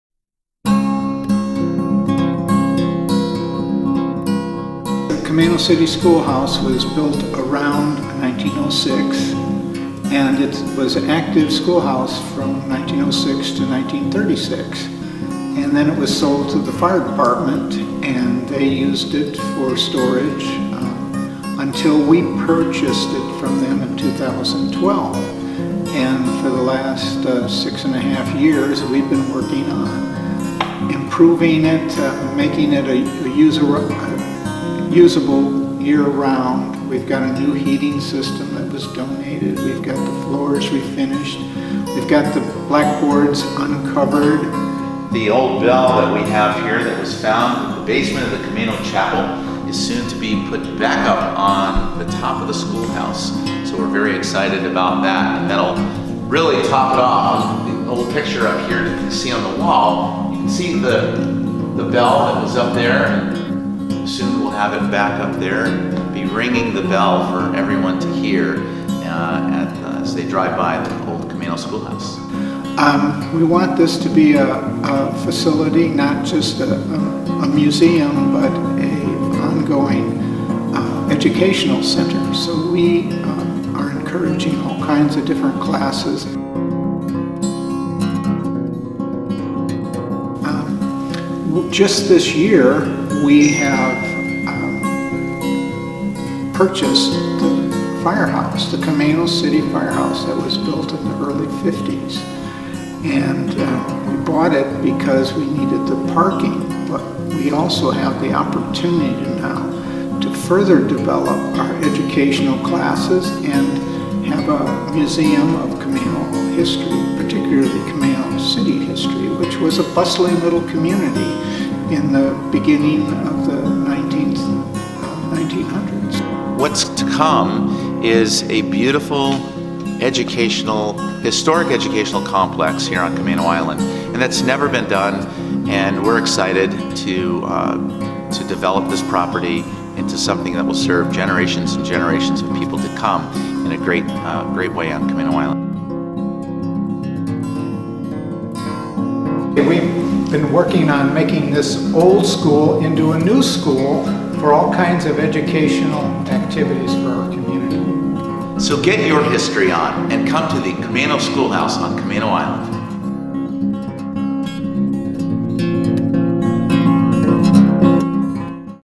The Story of the Camano City Schoolhouse
Learn about the old 1906 Schoolhouse on Camano Island that had been used as a storage building for the Fire Department until a small group of passionate locals decided to save the Schoolhouse and restore it into a new working Educational Center for the community.

Chuck Durland and Ed Bednarczyk share the history and vision for creating a historic educational complex on Camano Island, WA.

The Schoolhouse is one of the sites open to the public during the annual Stanwood Camano Historic Sites Tour in September. for details on this free, self-guided tour.